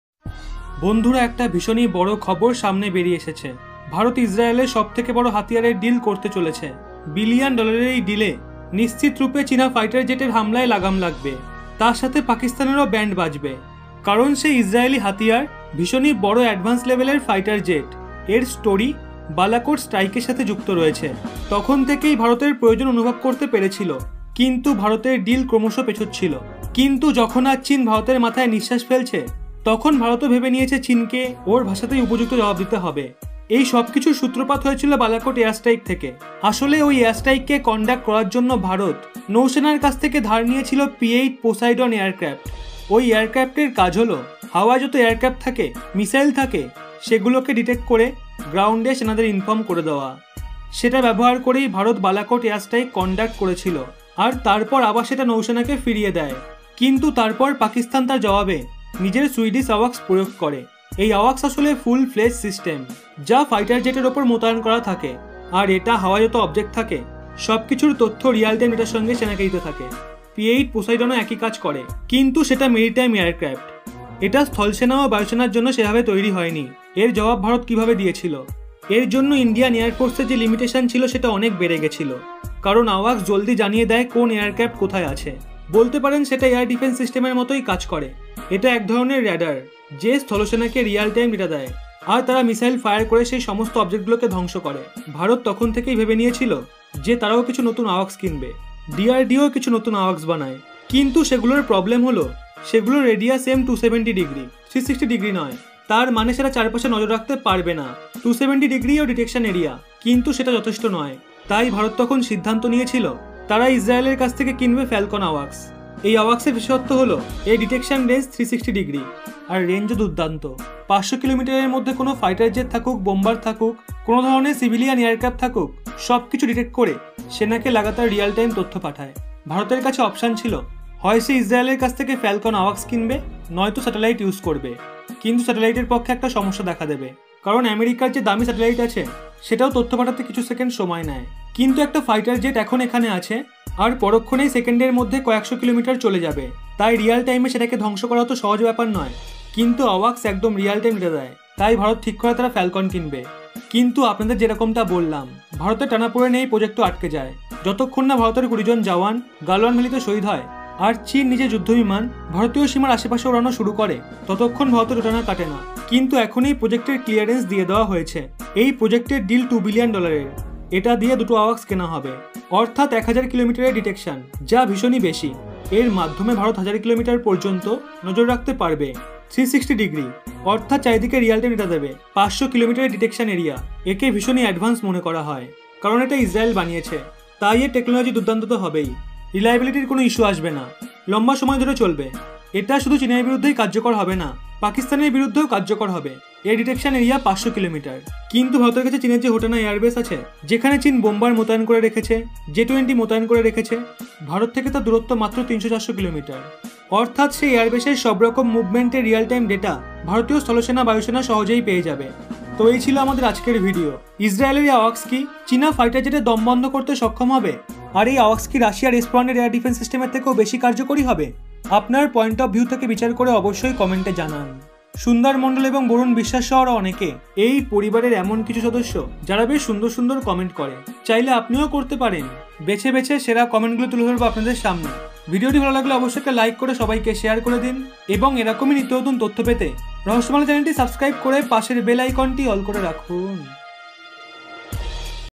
ভারতে আসছে ইসরায়েলের সবথেকে ভয়ঙ্কর হাতিয়ার//ভয়ানক বিপদের মুখে চীন
ভারতে আসছে ইসরায়েলের সবথেকে ভয়ঙ্কর হাতিয়ার//চীনকে শেষ করতে বদ্ধপরিকর ভারত ইসরায়েল